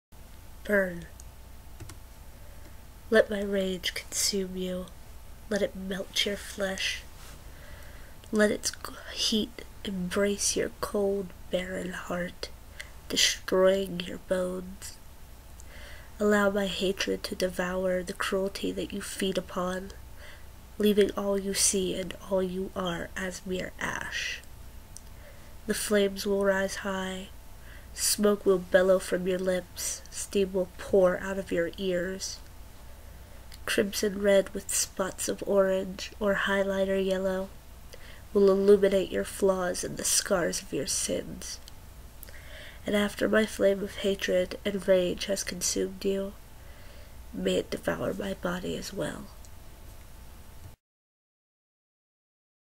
Burn: an original poem
My poem, my voice. Not my picture, found it on photobucket and it belongs to it's rightful artist. I'm just using it to pretty up my video and my poem.